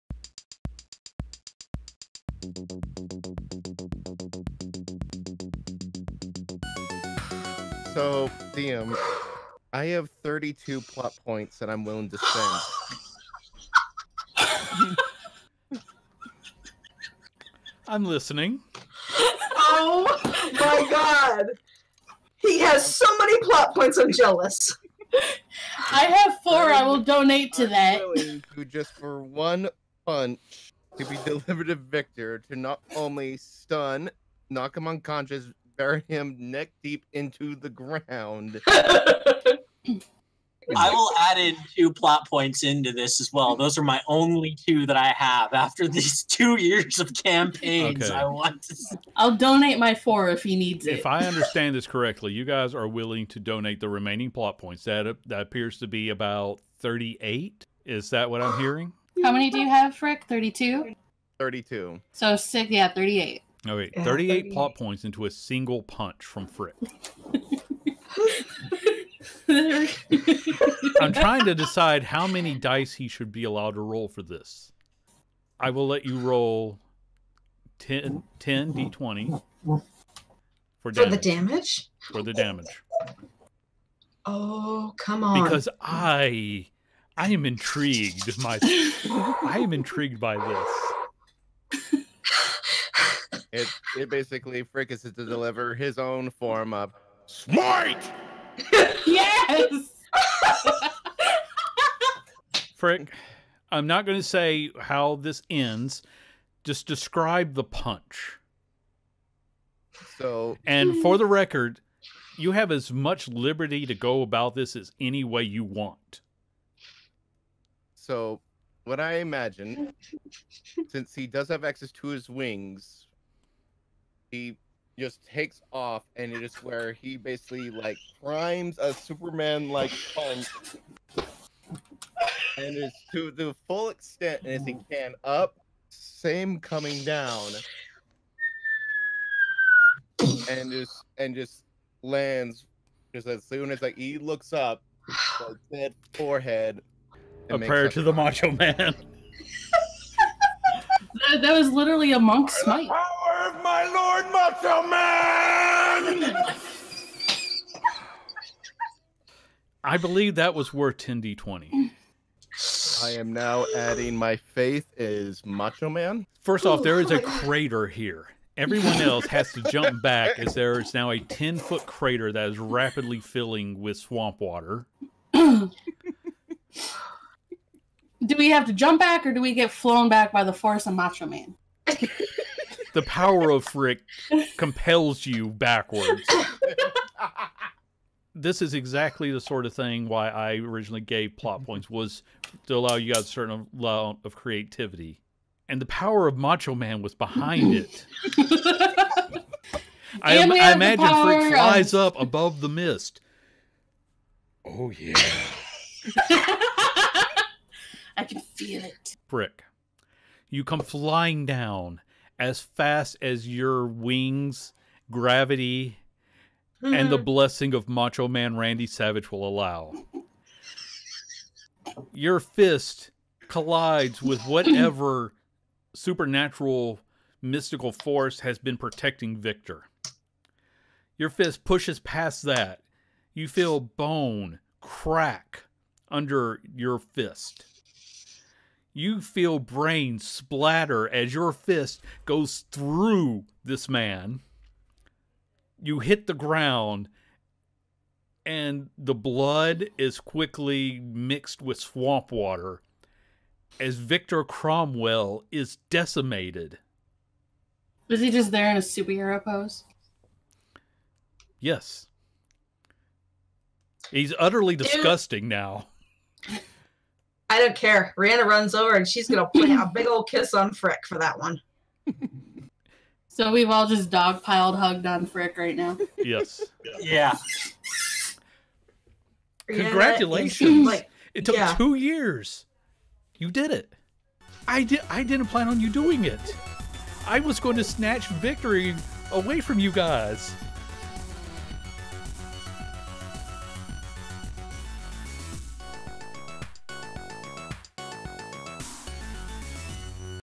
The Expendable Crew: Fall of Victor
The epic conclusion to an arch in our D&D game on the two years anniversary game.
Character minis thanks to heroforge.com snapshot ability.

We have a shop! You can get your own Warped Films Merch over

Barry Driscoll